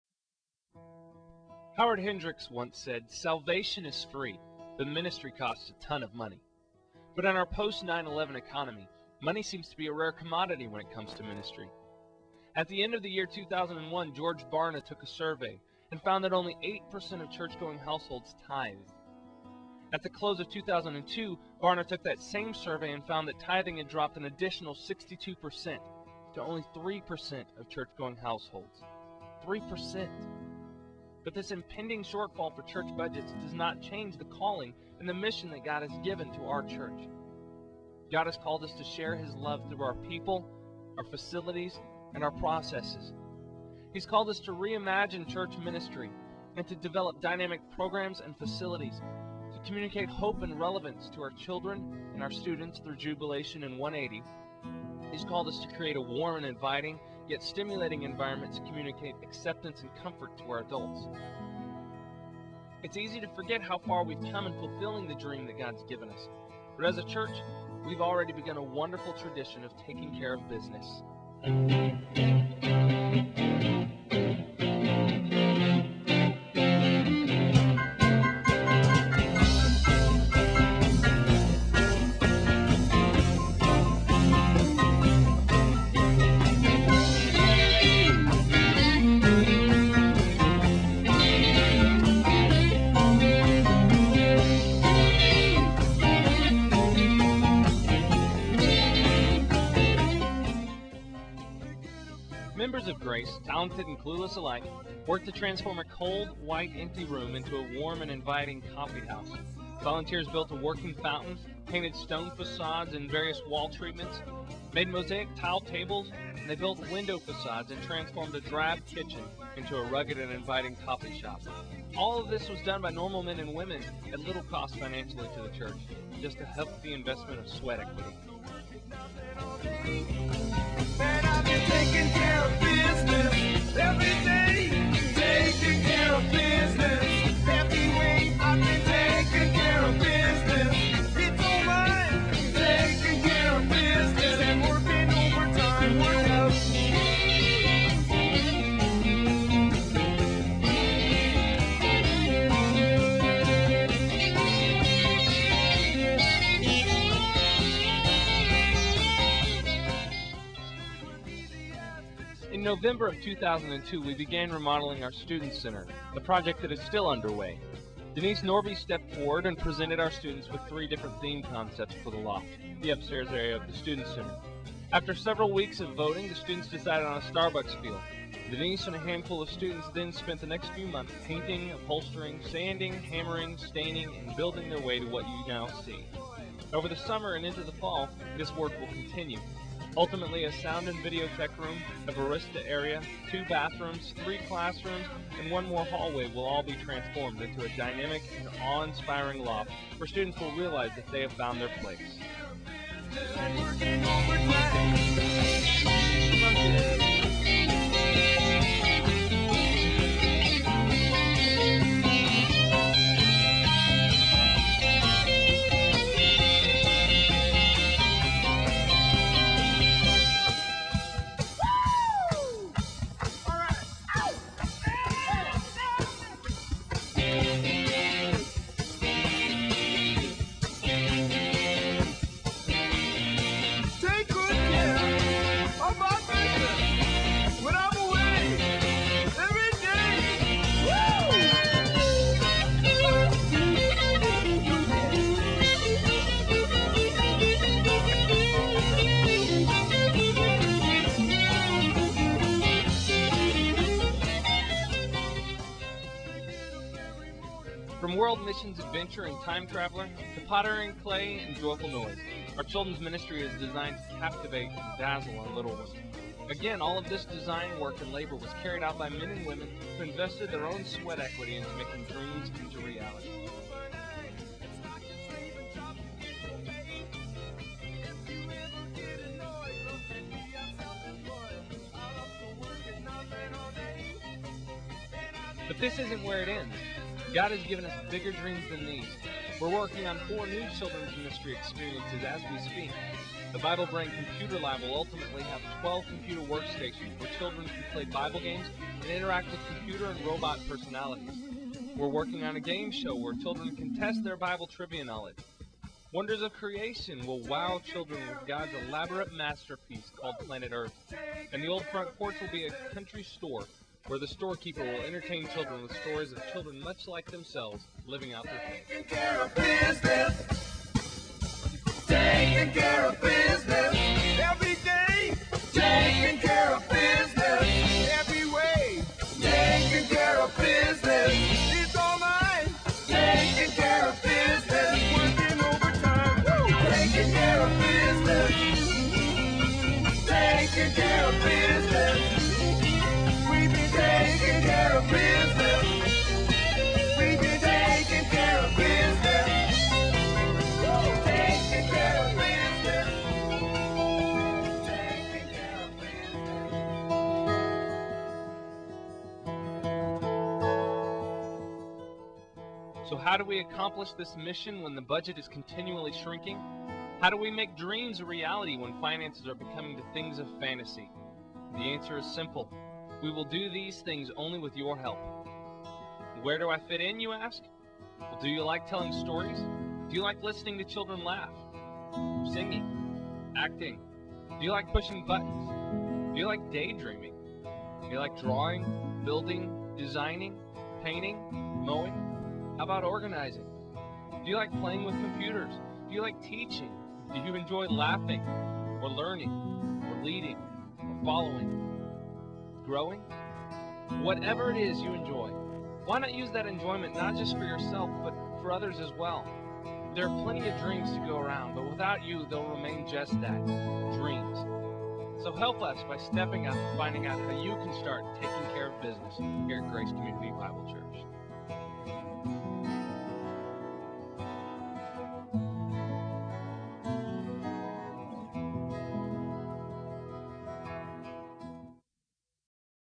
Grace CBC 2003 Town Hall Presentation
This is a presentation I produced when I was on staff at Grace Community Bible Church in Richmond, Texas. We had our annual "town hall" meeting & we wantedt to showcase some of the projects we had completed & those we were still working on. This is the only copy I have of it & it has been compressed so much that it's lost quite a bit of its original quality, but you get the idea.